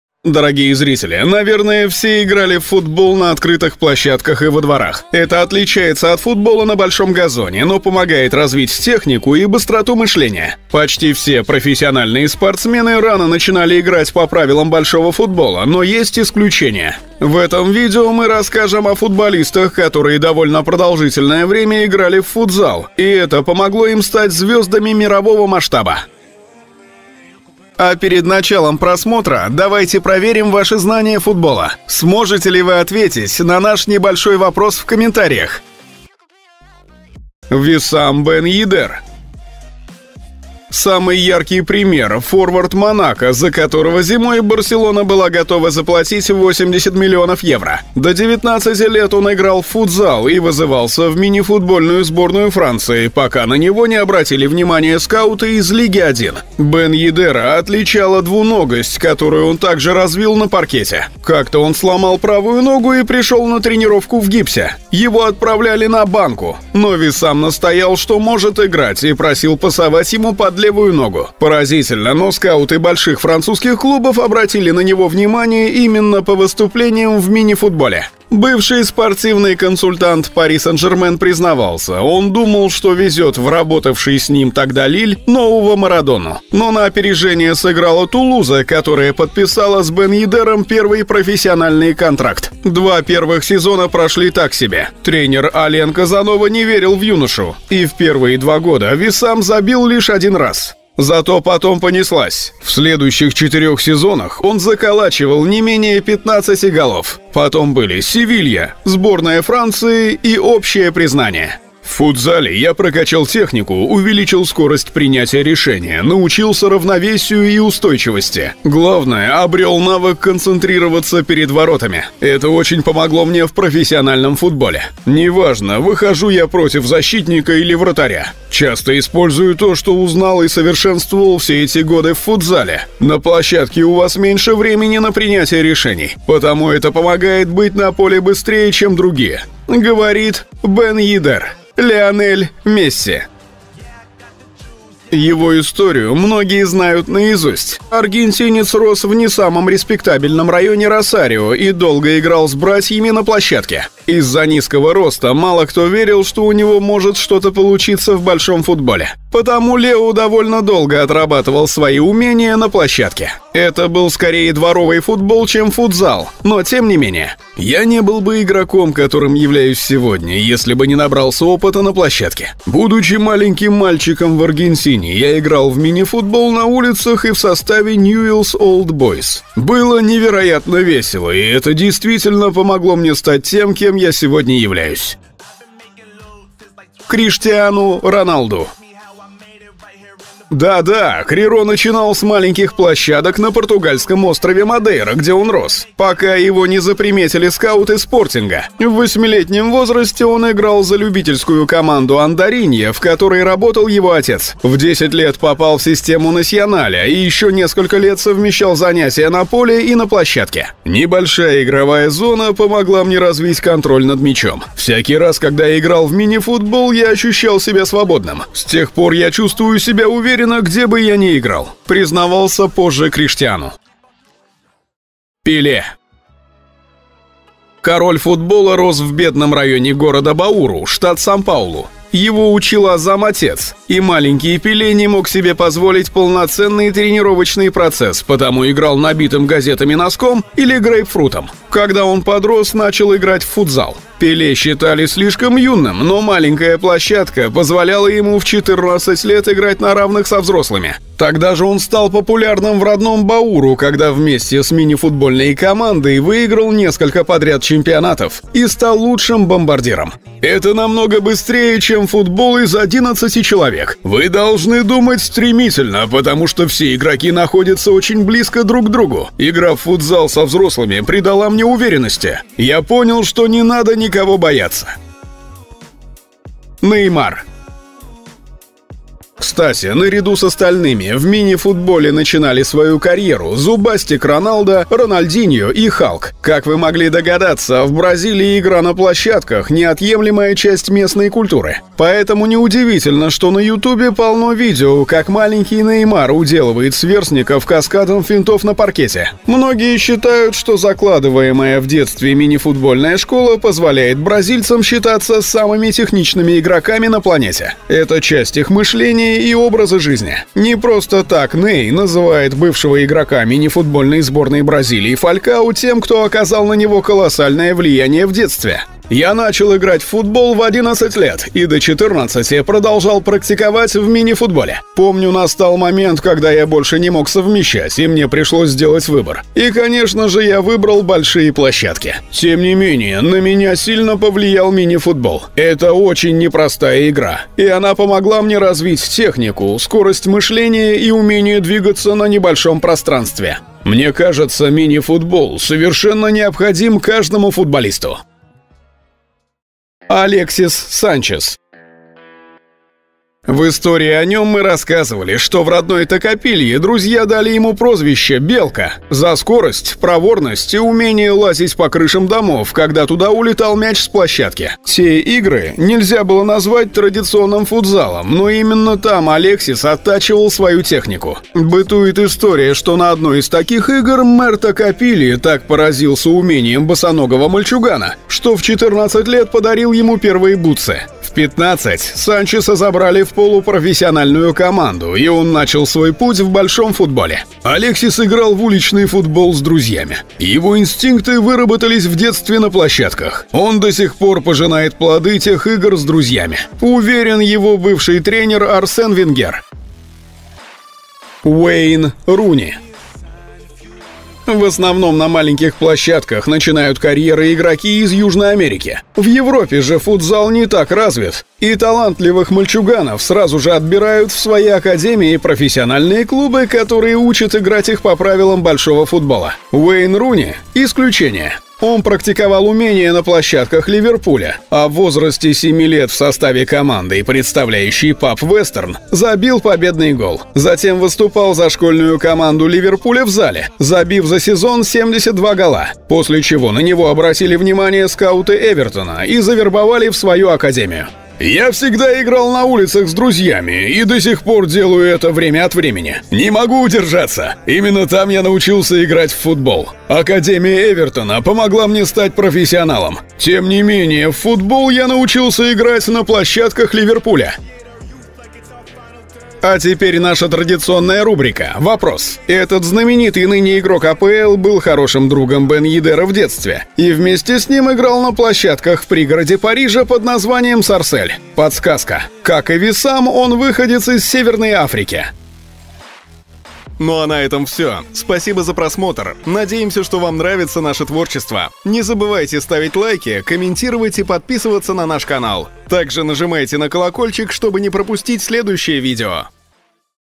ОНИ НАЧИНАЛИ СВОЮ КАРЬЕРУ В ФУТЗАЛЕ. ТОП-ФУТБОЛИСТЫ, КОТОРЫЕ НАЧИНАЛИ С ФУТЗАЛА - GOAL24
В этом видео мы расскажем вам о футболистах, которые довольно продолжительное время играли в футзал, и это помогло им стать звёздами мирового масштаба.